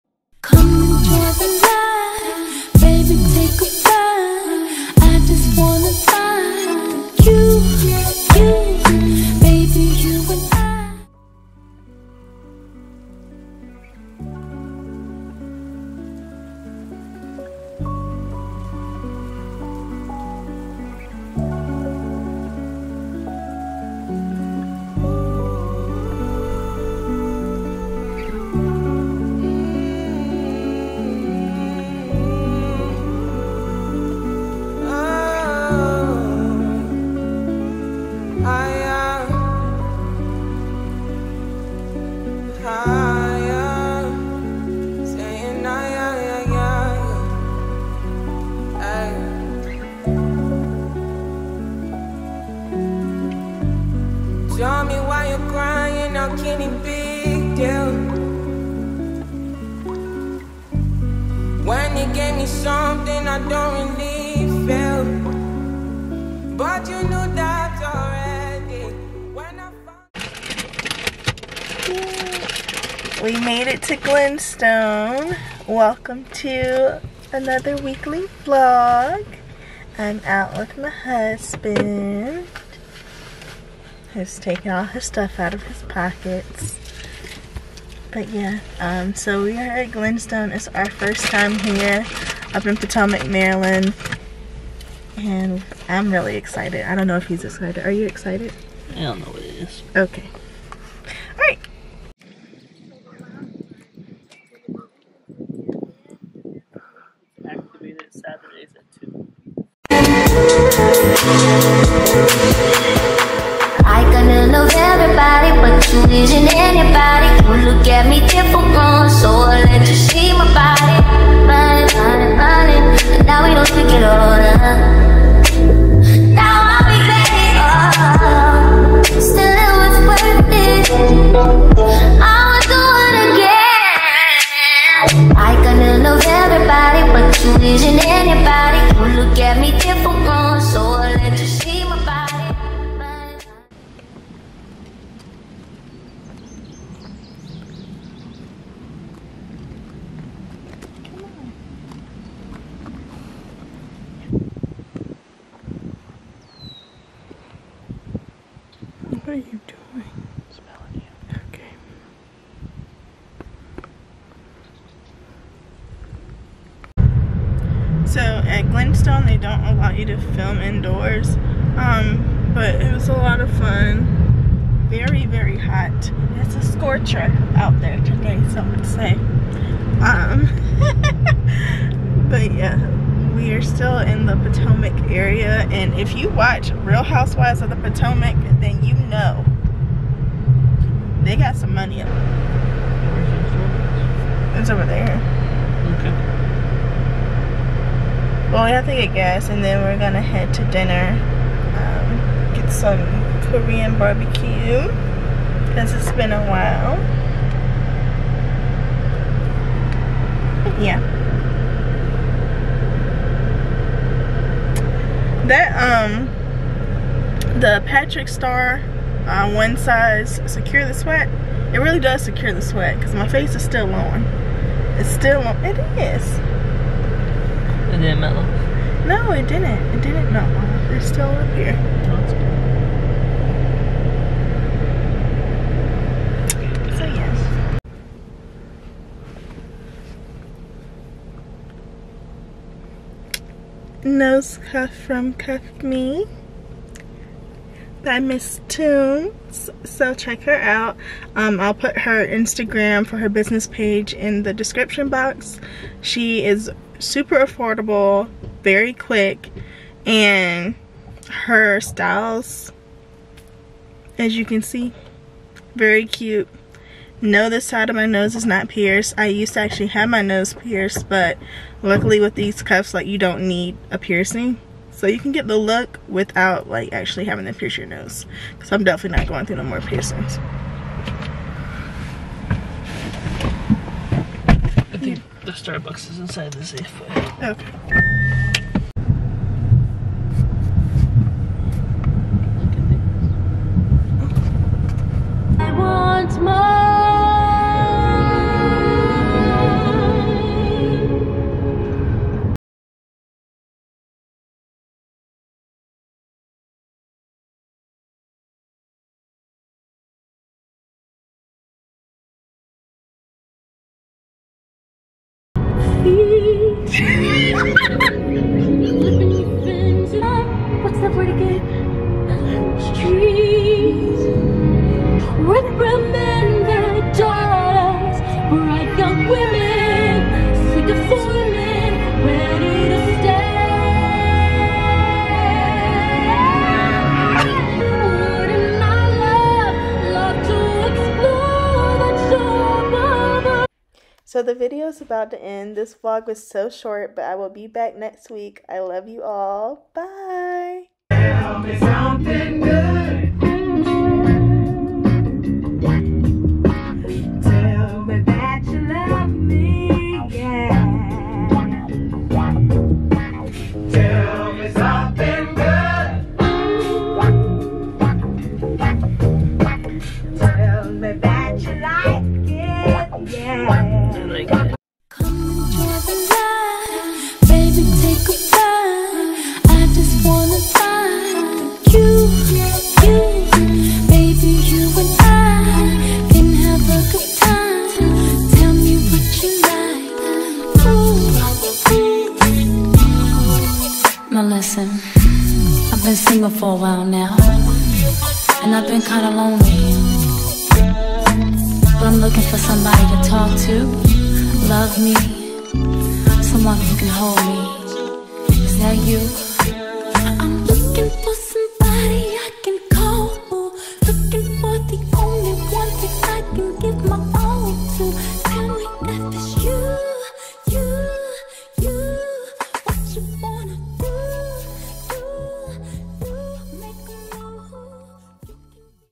WEEKLY VLOG: Quick Sunday Trip
Hi family and friends! This one is a short one, still wanted to share our day. If you haven't visited the Potomac area, I highly recommend, it's stunning. ALSO if you haven't seen the Little Mermaid you're missing out!

Nose Cuff: @cuffmee on Instagram
Glenstone (Potomac, MD)
So Korean (Centreville, VA)